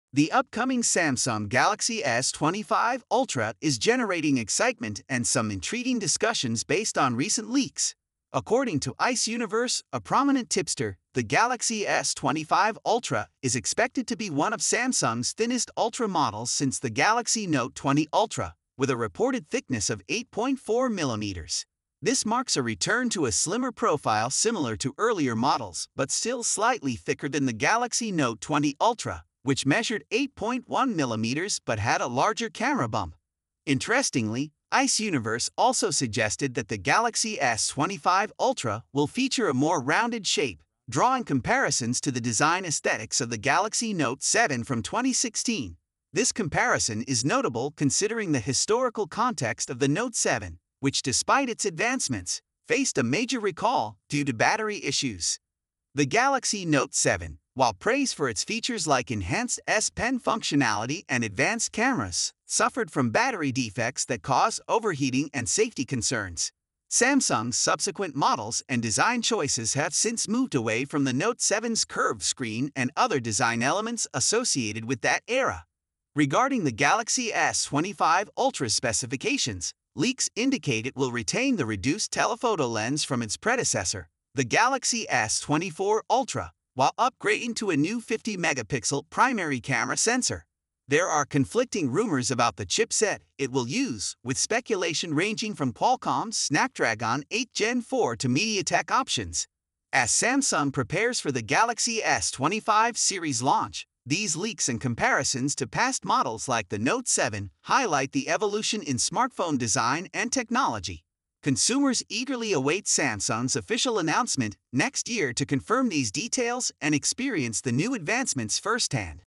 Samsung Galaxy S25 Ultra - Shocking Design !!!
The Samsung Galaxy S25 Ultra is generating buzz with its sleek design and advanced features! Leaked by tipster Ice Universe, the S25 Ultra is expected to be one of Samsung's thinnest Ultra models at 8.4 mm, drawing design inspiration from the Galaxy Note 7 with a more rounded shape. Anticipate a powerful 50MP primary camera and the potential use of Qualcomm's Snapdragon 8 Gen 4 chipset. Dive into the latest leaks and rumors about Samsung's upcoming flagship, set to launch next year! GalaxyS25Ultra SamsungLeaks SmartphoneInnovation TechNews MobileUpdate MRA